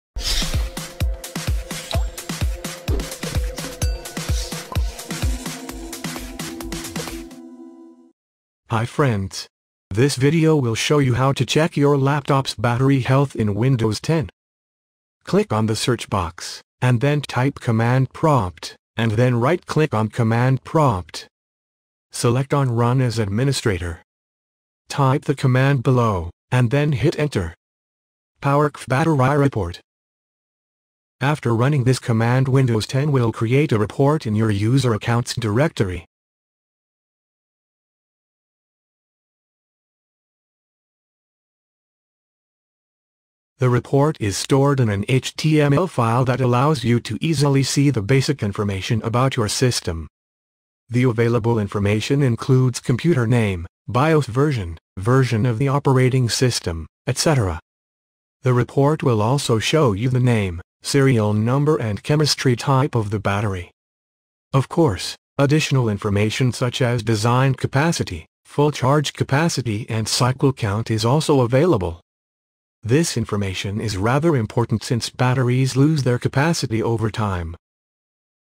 How to check laptop battery health Windows 10 (HP/Dell/Acer/Asus/Levono)
Hi friends! This video will show you how to Check your Laptop's Battery Health in Windows 10.

Click on the Search box, and then type 'cmd,' and then Right click on 'Command Prompt.'

Select on 'Run As Administrator.'

Type the command below, and then Hit Enter.

powercfg /batteryreport

After running this command Windows 10 will create a report in your user account’s directory.

The report is stored in an HTML file that allows you to easily see the basic information about your system. The available information includes computer name, BIOS version, version of the operating system, etc.

The report will also show you the name, serial number and chemistry type of the battery.